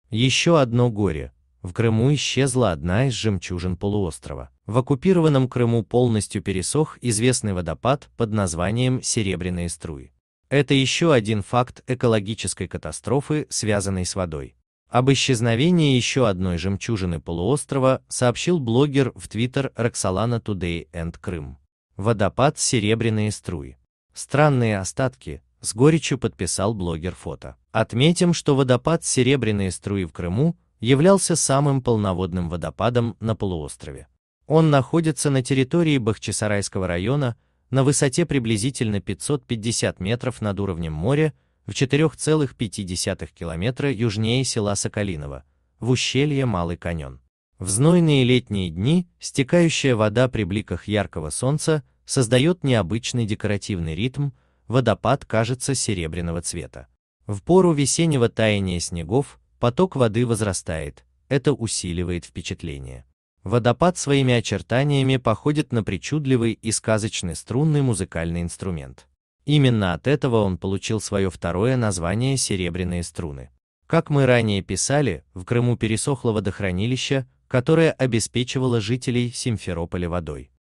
Еще одно горе: в Крыму исчезла одна из жемчужин полуострова.
Жизнь Крым Подробней на В оккупированном Крыму полностью пересох известный водопад под названием Серебряные струи. Это еще один факт экологической катастрофы, связанной с водой. Об исчезновении еще одной жемчужины полуострова сообщил блогер в Twitter Roksolana Today & Крым. "Водопад Серебряные струи. Странные остатки", – с горечью подписал блогер фото. Отметим, что водопад Серебряные струи в Крыму являлся самым полноводным водопадом на полуострове. Источник